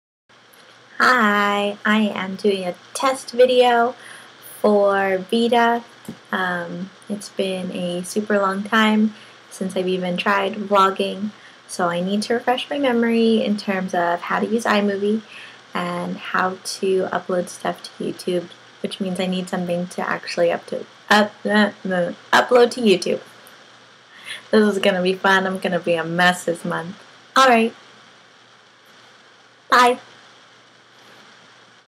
VEDA 2013 Prep: Testing, 1, 2.
Hi, my name is Sheryl and I don't know how to vlog. Here's to hoping I figure it out some time during my VEDA journey. In the meantime, please enjoy the awkwardness that is this VEDA test video! (: